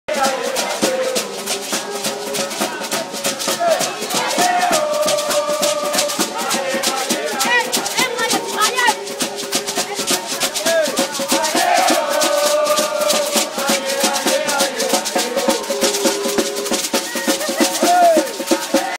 Carnaval école Marcel CACHIN
Défilé carnaval des écoles de Choisy le roi
13/04/2013